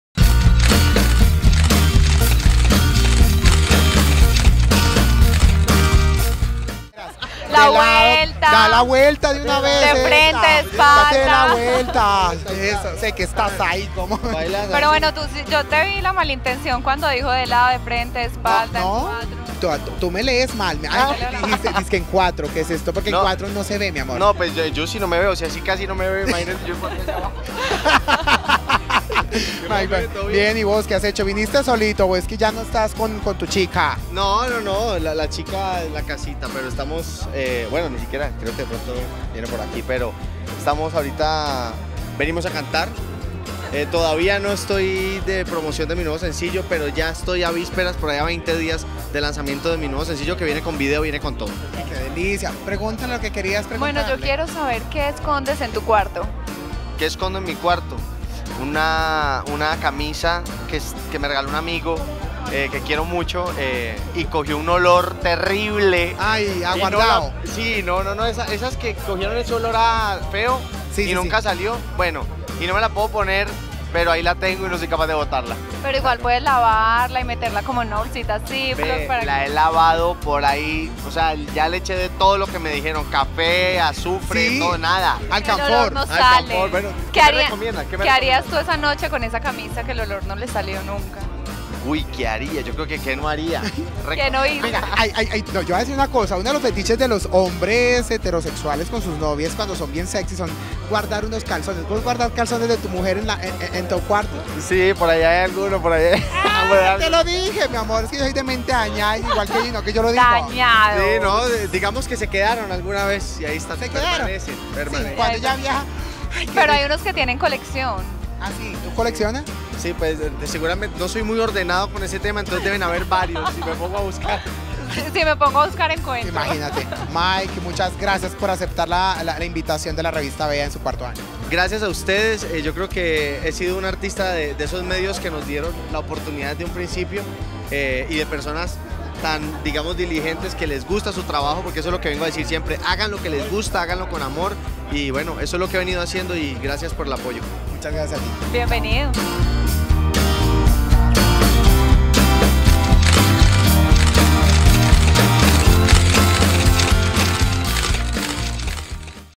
Pink Carpet - Mike Bahía en el CuartoAniversarioVea - Revista Vea
Mira la entrevista a Mike Bahia en la ‪#‎PinkCarpet‬ de nuestra fiesta de ‪#‎CuartoAniversarioVea‬. Descúbre todos los detalles de esta gran celebración en nuestra edición impresa. ¡No te lo pierdas!